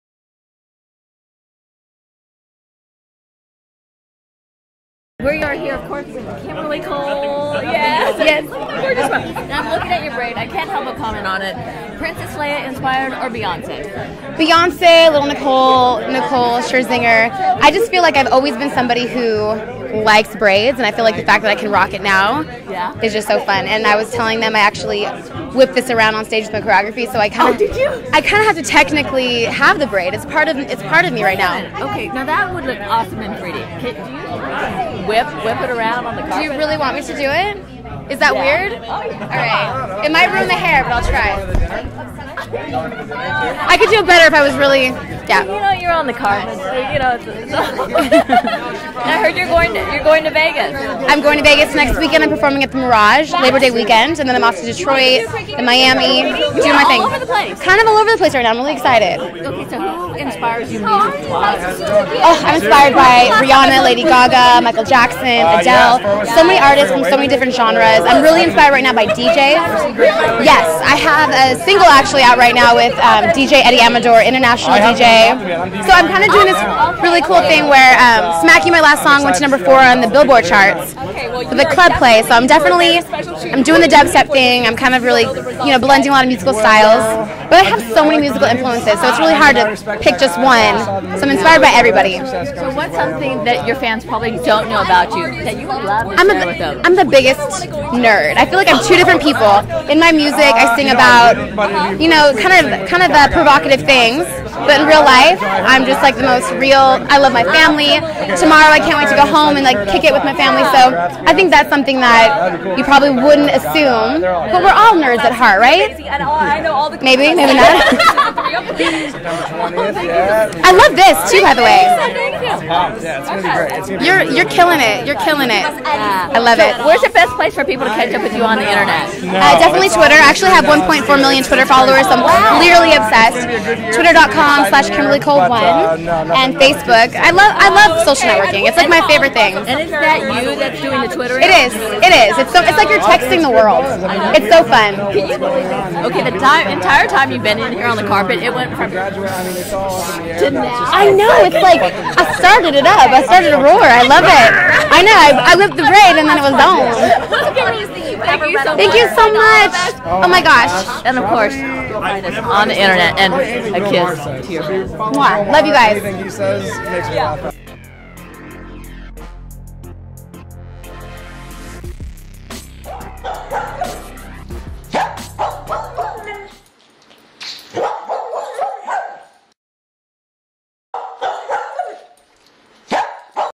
Kimberly Cole in 3D
Artist Kimberly Cole interviewed by Joyce Chow of MBN and Newsvideoweb. 4th Annual House of Hype Music Awards Event for Esquire Magazine Hot 100 Dinner and House of Hype Post Awards Party in anaglyph 3D.